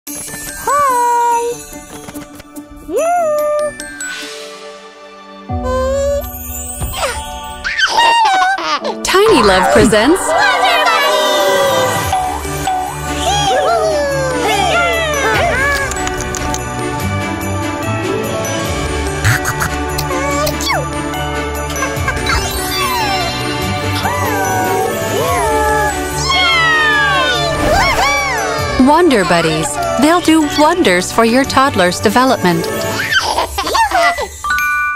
Introducing Wonder Buddies™! Developmental Toys for 1-Year-Olds
Especially created for one-year-olds, the Wonder Buddies™ will become your baby's first playmate and encourage learning–the fun way. Featuring seven activities that match the skills and abilities of children ages one and up, these developmental toys are designed with smart sensors, making playtime interactive and educational.

Wonder Buddies™ are made for both independent and guided play. Your toddler will develop communication, cognition, creativity, emotions and more, all while having fun fun fun.